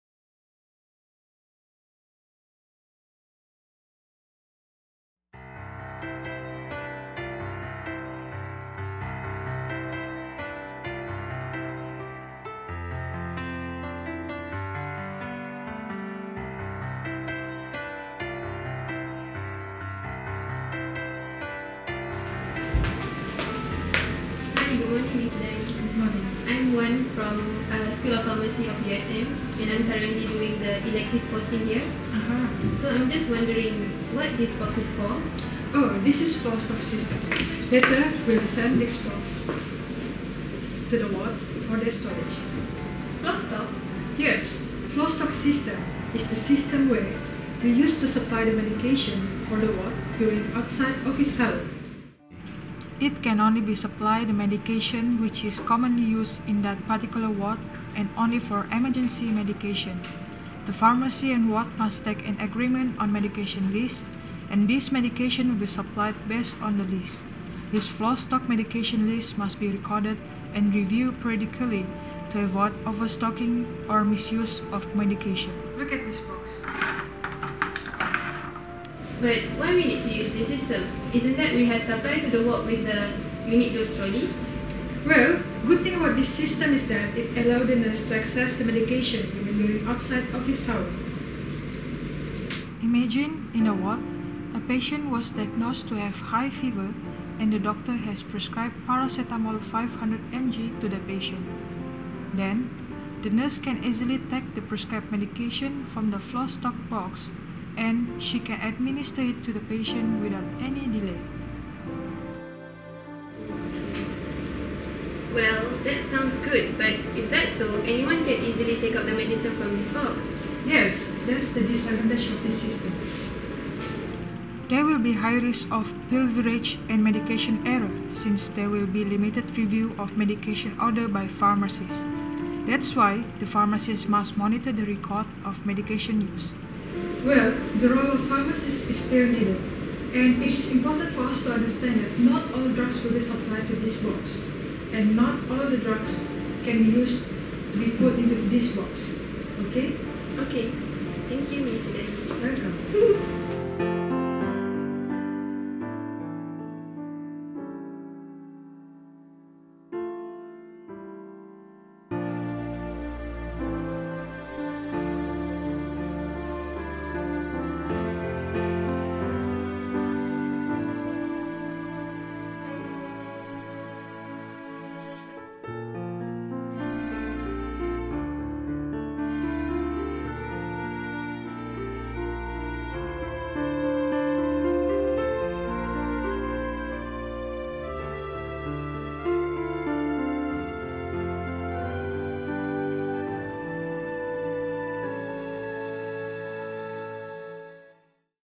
Floor Stock System
A informative and excellent video on Floor Stock System prepared by our Fourth year student as part of their group task for Satellite Pharmacy Clerkship.

Prepared by Satellite Pharmacy group J (2014/15).
CHAI WAN JUN
ELVIN GUAN KHAI XIAN
FATIN SYAHIRAH BINTI JASNI
MOHD SHAFIQ BIN MOHD SARIF
NORHIDAYU SOON GIN YEE
NURUL SYUHADAH BINTI ZAKARIA
SITI MARINI BINTI RAZALI
TAN MEI FANG
TAN WEI LIANG
WAN FATIMAH BINTI RAMLI
YASMIN BINTI  ABDUL AZIZ